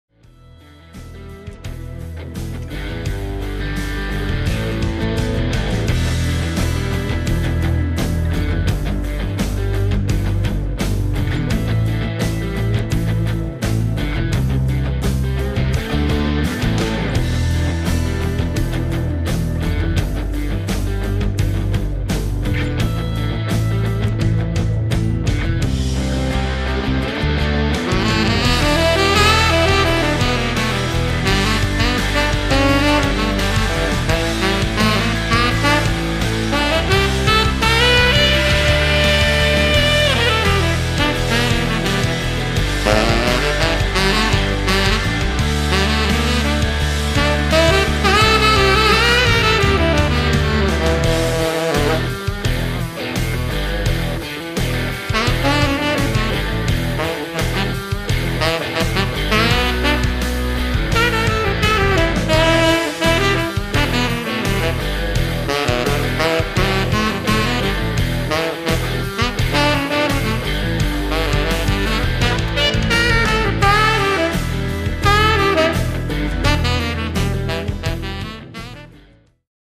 DIRTY ROCK/ BLUES SOLO SAX
Crudo rock blues con le chitarre distorte sotto appena tornato a casa dal lavoro ! dirty and rocking sax solo !ovvero quello che sento dentro e anche di piu'..